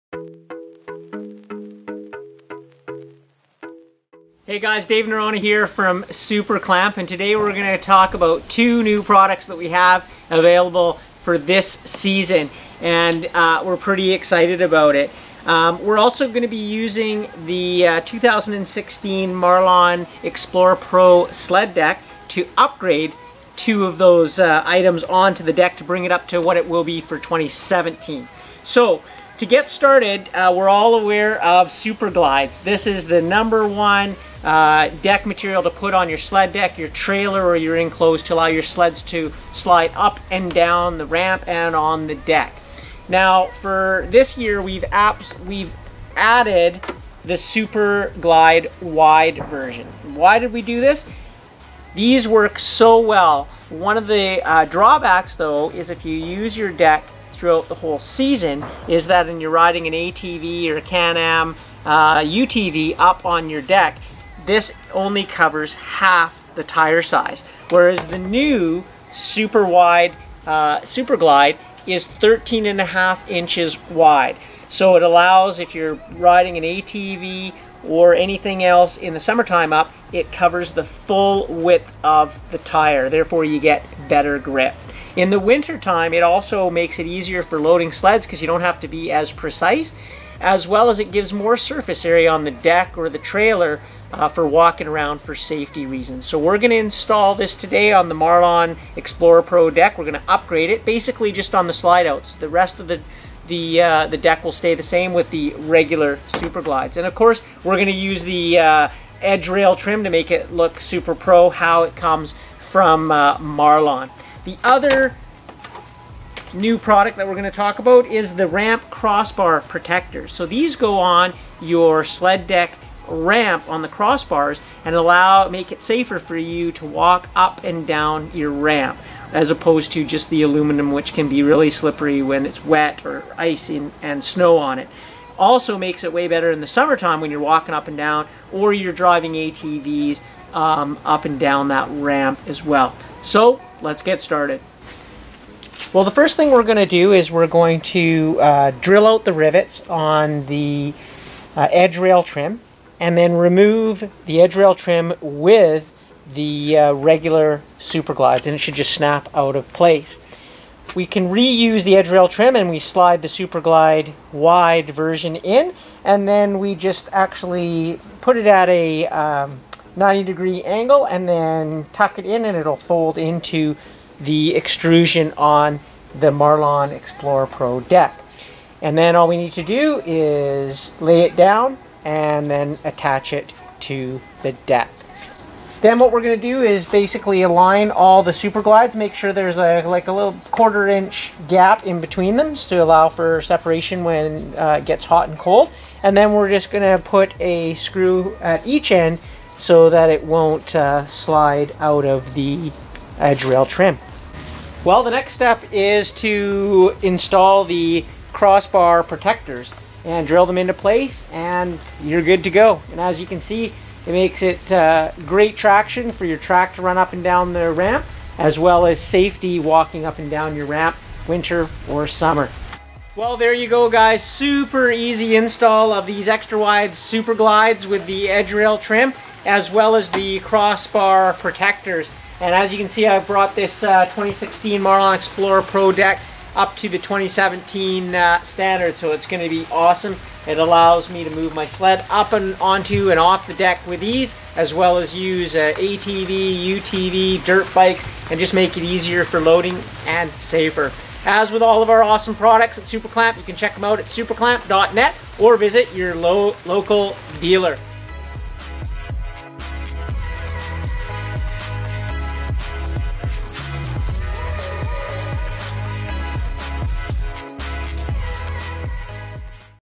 Superclamp.net Wide Superglides and Ramp CrossBar Protectors to the Marlon Xplore Pro Sled Deck
Ski-Doo Backcountry Expert Dave Norona takes us through upgrading his Marlon Xplore Pro Sled Deck with Superclamps new Wide Superglides and CorssBar Protectors to make it easy loading and unloading sleds and atv's as well as making it safer for walking on the deck and up and down the ramp! Made in Canada!